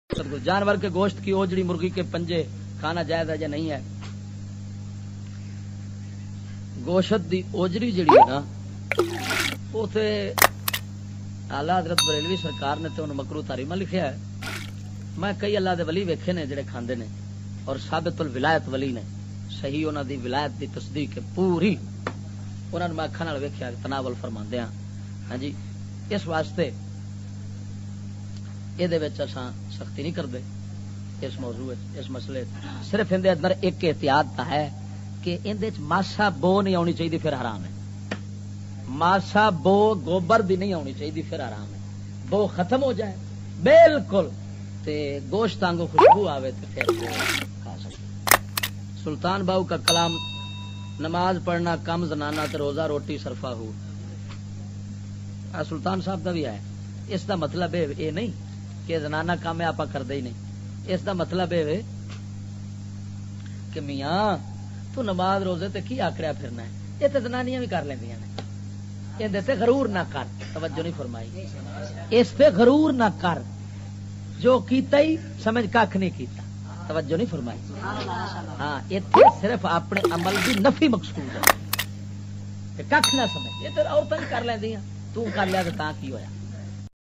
ojhri aur murgi ke panje khana kaisa hai @Mufti Fazal Ahmad Chishti New Bayan/fazal ahmad chishti
ojhri aur murgi ke panje khana kaisa hai @Mufti Fazal Ahmad Chishti New Bayan/fazal ahmad chishti اوجری اور مرغی کے پنجے کھانا جائز ہے؟